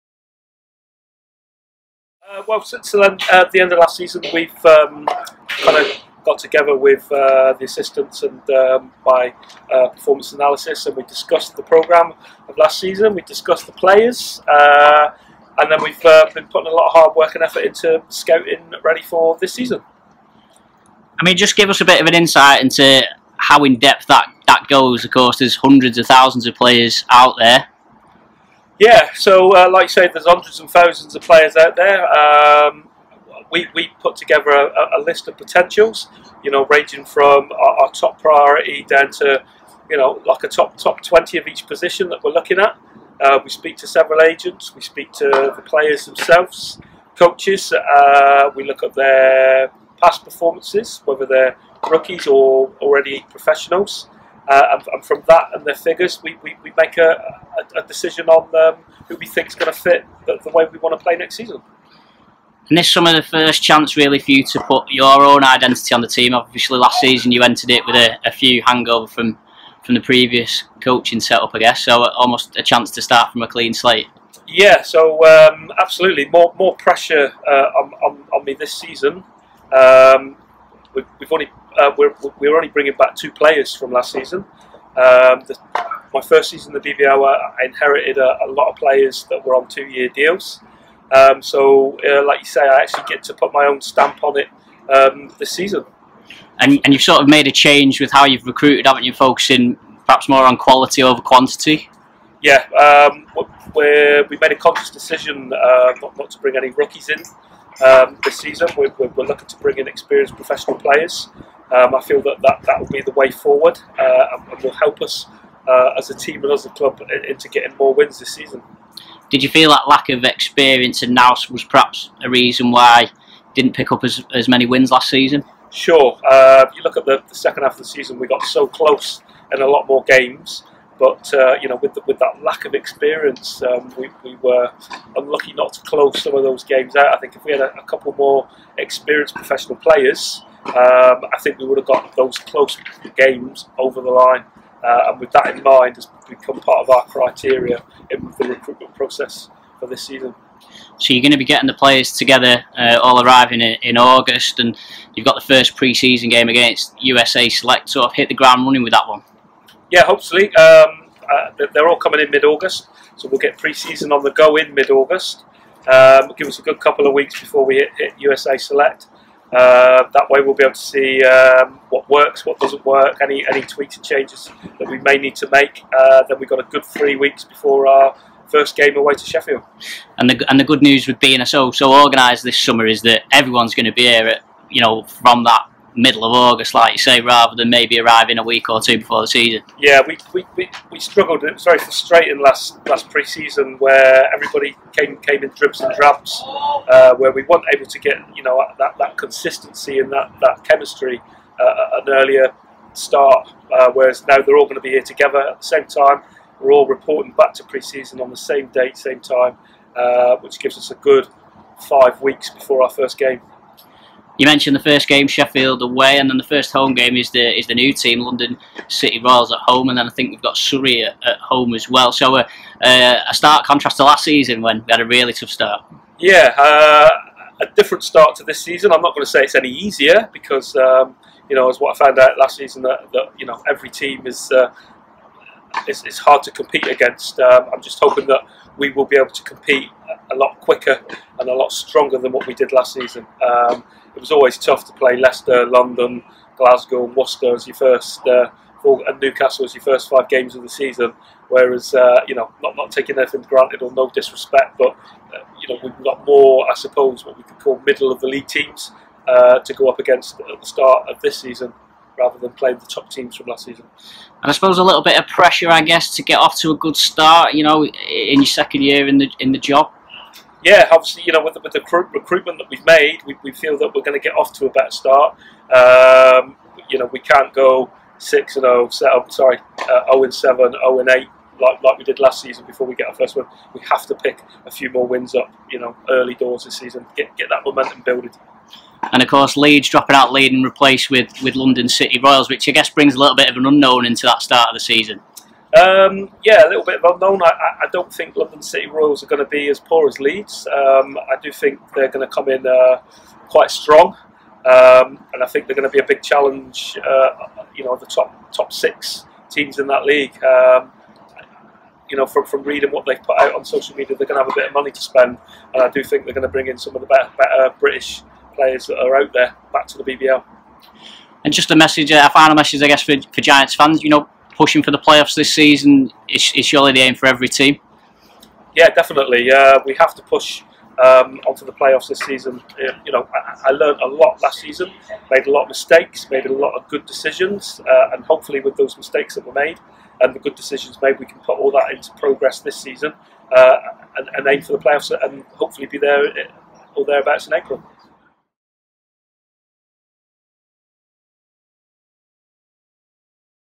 Danny Byrne discusses off-season progress | Giants TV
Manchester Giants head coach Danny Byrne discusses his recruitment strategy and provides an insight in to what goes on behind the scenes for the coaching staff during the off-season.